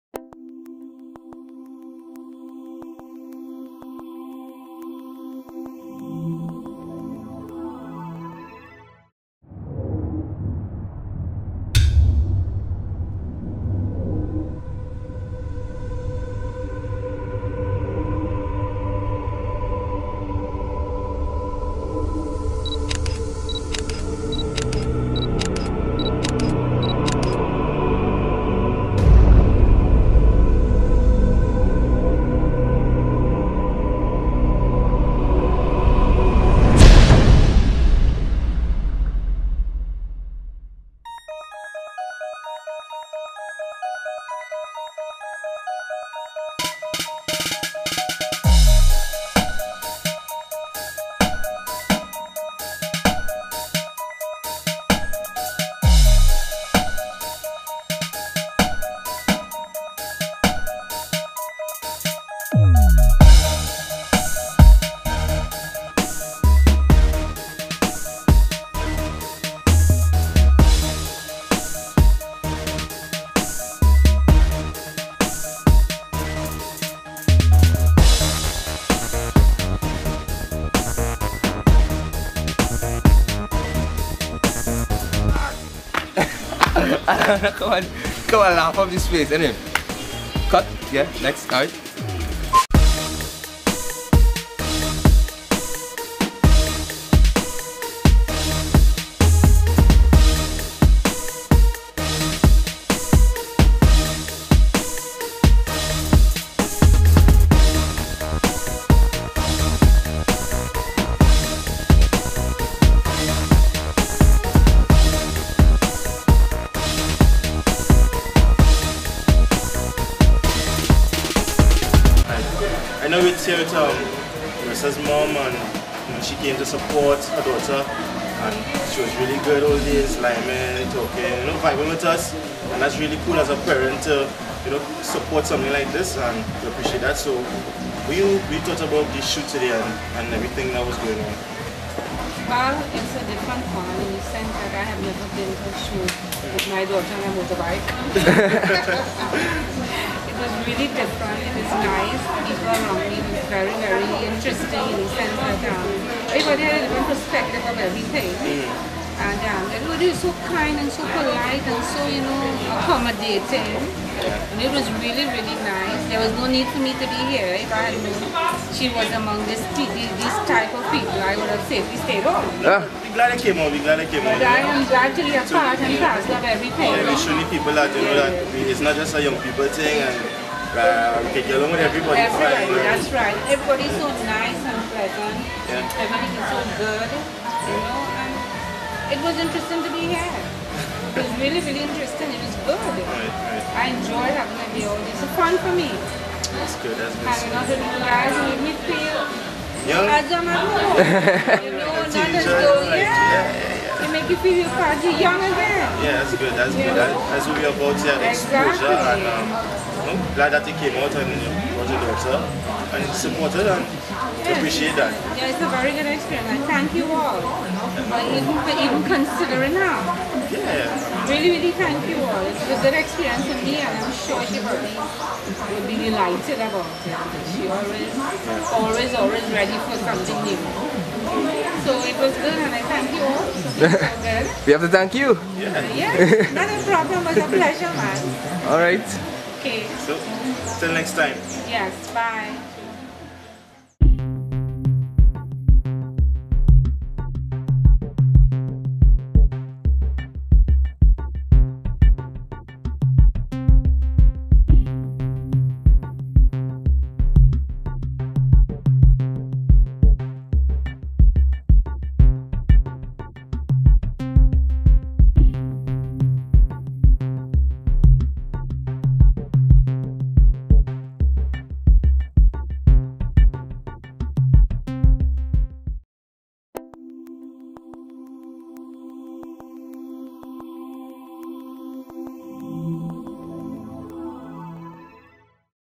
Exposure
3 elements that makes the Expo.Sure Event.
Photography
Videography
Music

Expo.Sure Team
Kit Israel
Justyn Dion Mayers
Alana Jadoo
J K Mayers

Make up.
Charle Khadiya Roberts
Onika Edwards

Models.
Marisa Mahadeo
Victoria Mary
Dwayne Hinds

Special Thanks to: Anyse Joachim and Ashanna Arthur